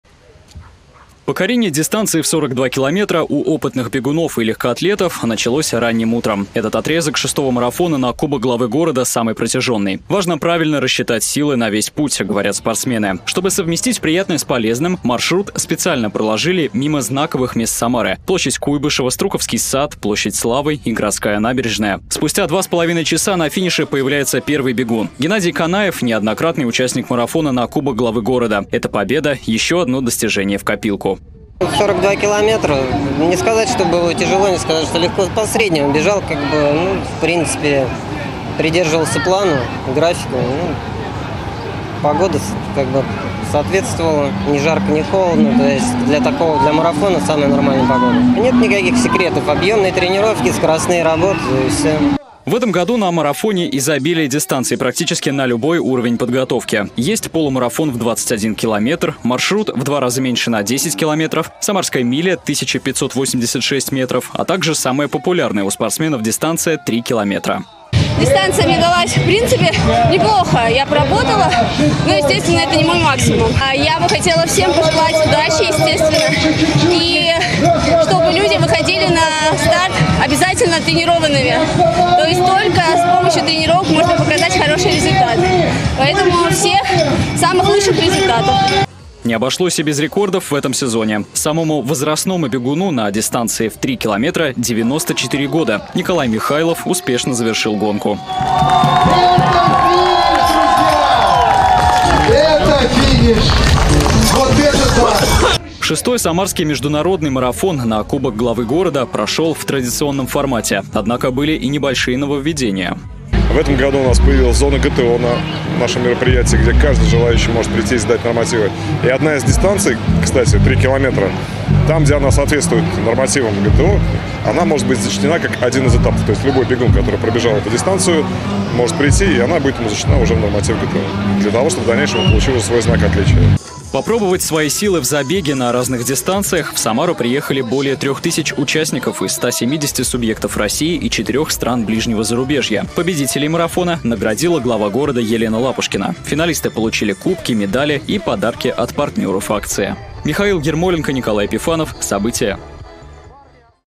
В Самаре в марафонском забеге приняли участие более трех тысяч спортсменов
Больше трех тысяч человек из 170 субъектов России и стран ближнего зарубежья приняли участие в марафоне на кубок главы города. Международное легкоатлетическое соревнование прошло в Самаре 6-й раз. В этом сезоне не обошлось без рекордов - самым возрастным участником забега стал 94-летний Николай Михайлов. Остальные подробности - в нашем материале.